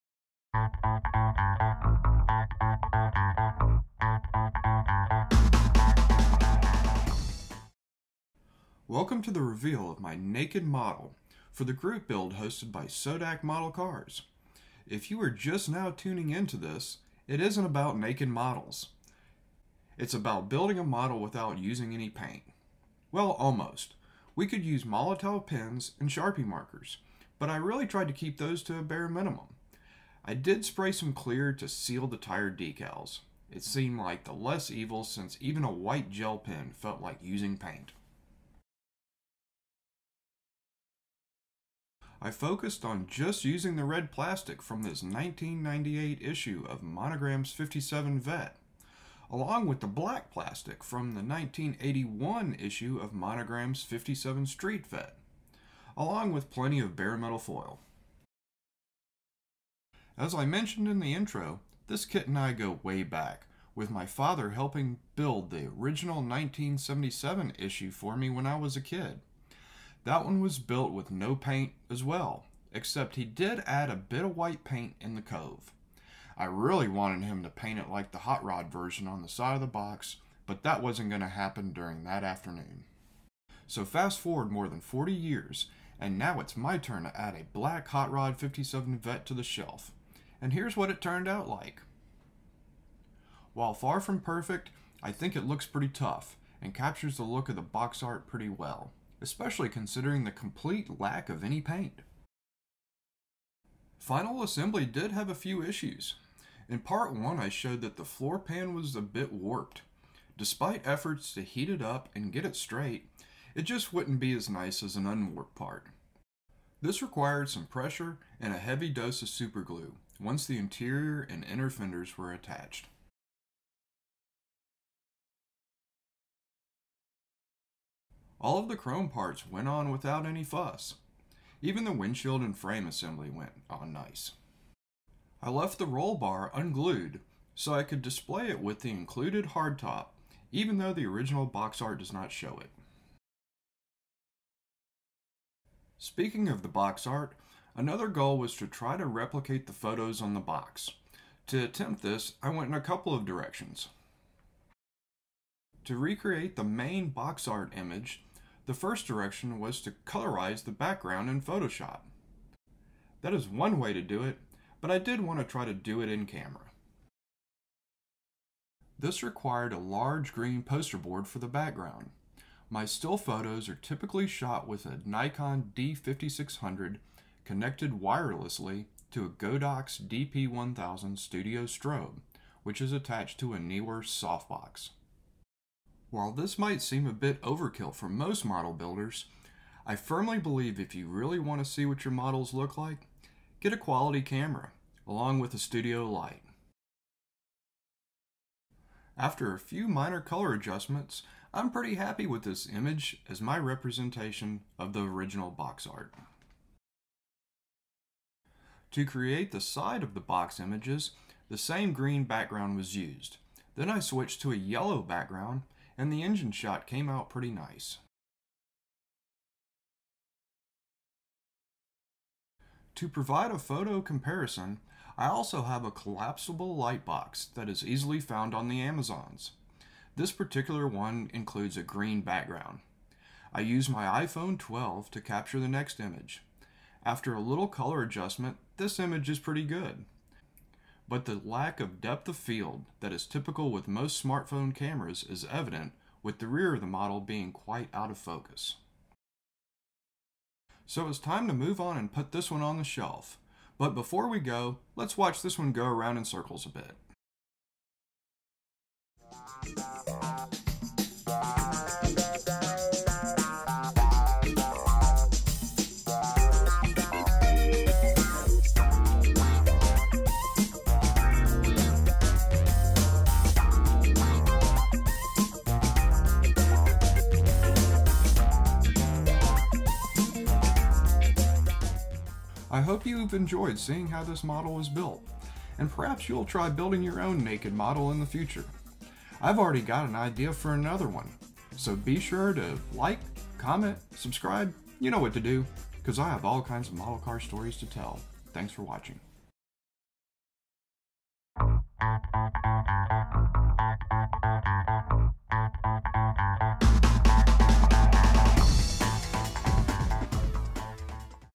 Naked Model Revealed! Just like the box art? Also a photography how-to.
Building a Monogram ’57 Street Vette kit issued in 1981. In this series, the plan was to replicate that box art with some basic supplies and at the same time, use NO PAINT.

With some creative use of kit variations and a bit of plastic surgery, this project should be possible by anyone regardless of their skill level. The techniques shown here could be applied to a variety of model car subject matter once the builder has researched the various color variations that a kit has been molded in.

This time check out the finished model and I explain what I did to recreate the original 1981 box art images. Follow along to see how I compare two very different camera and lighting set ups.